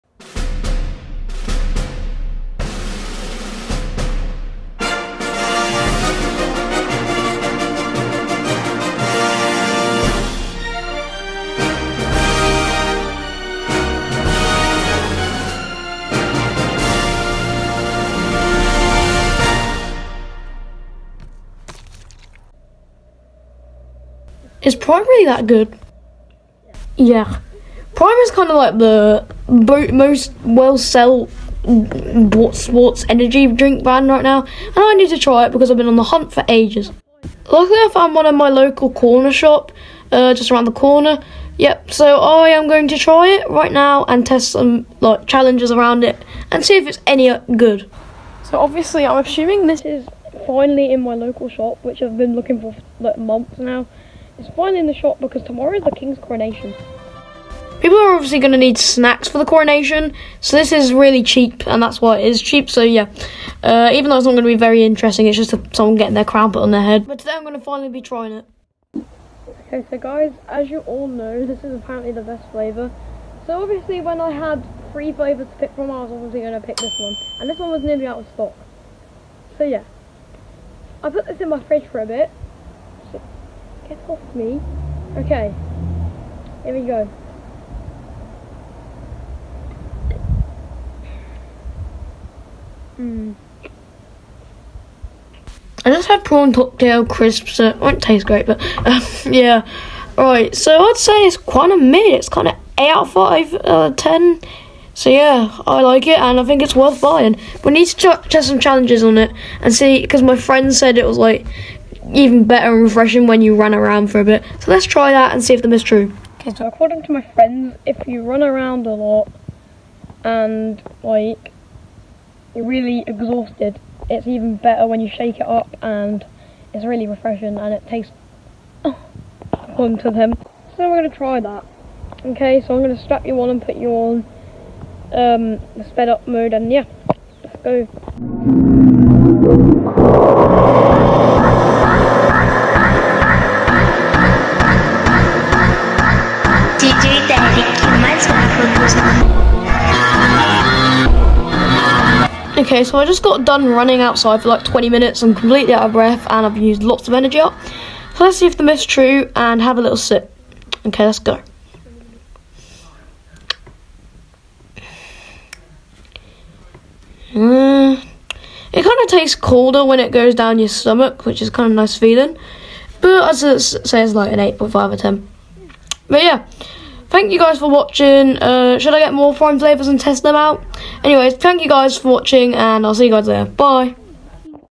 BRUTALLY HONEST PRIME REVIEW
I tried prime for the first time. P.S: it’s sexy